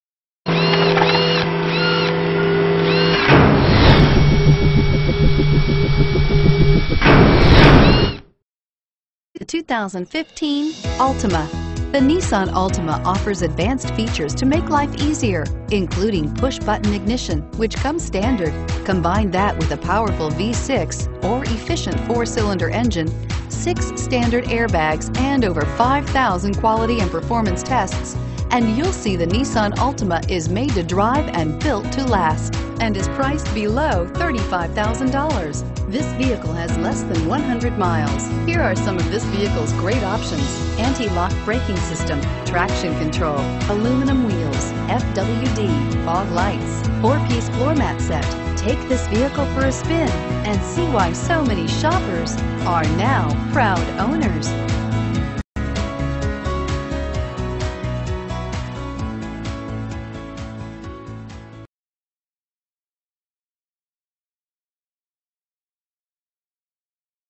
2015 Nissan Altima Bourne MA N13193
2015 Nissan Altima 3.5 SL

Pick up this Pearl White 2015 Nissan Altima, available today at Nissan of Bourne. Featuring Automatic transmission, it has 7 miles. This could be the one you've been searching for.